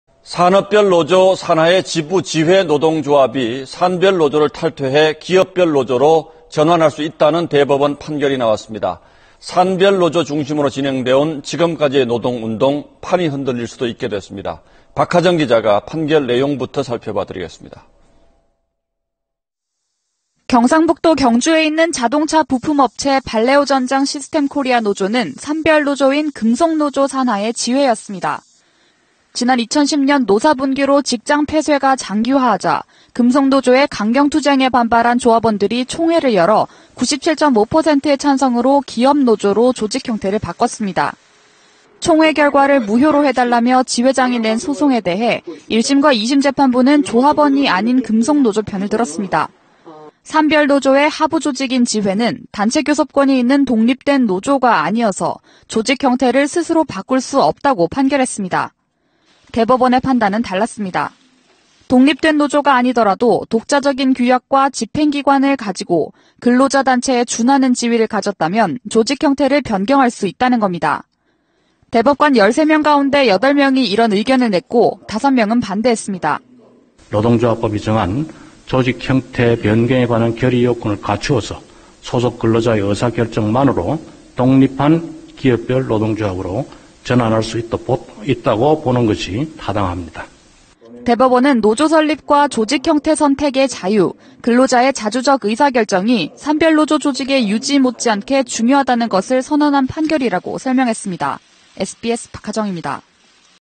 대법 "산별노조 탈퇴, 기업 노조 전환 가능" / SBS
산업별 노조 산하의 지부, 지회 노동조합이 산별노조를 탈퇴해 기업별 노조로 전환할 수 있다는 대법원 판결이 나왔습니다. 산별 노조 중심으로 진행돼 온 지금까지의 노동운동, 판이 흔들릴 수도 있게 됐습니다.